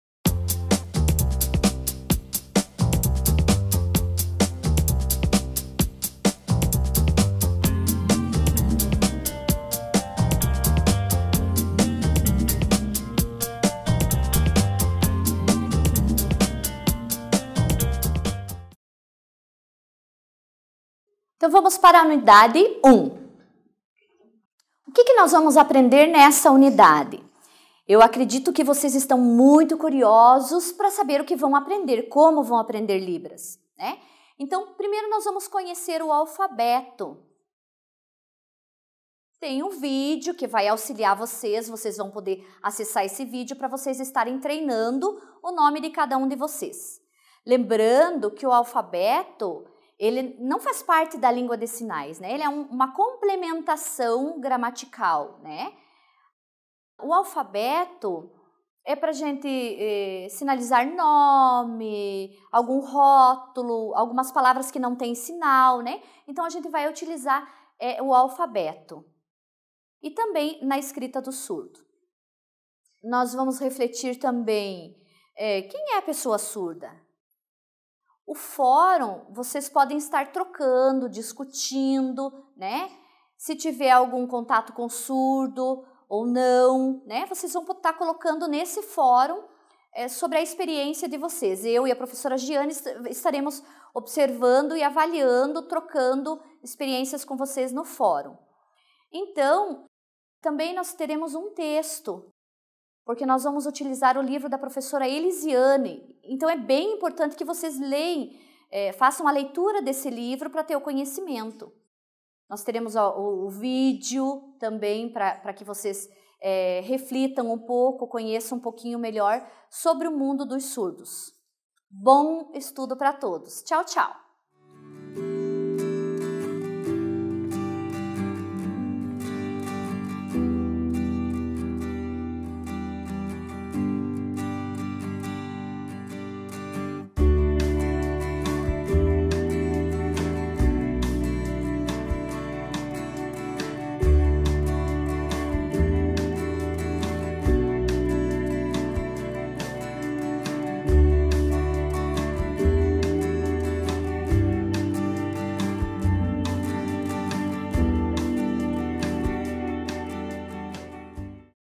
[PED] Libras - Unidade 1
Tema Unidade 1 na disciplina de Libras com as professoras Irene Stock e Jiane Ribeiro Neves Cwick (TILS). Curso EaD de Licenciatura em Pedagogia da Universidade Estadual do Centro Oeste do Paraná.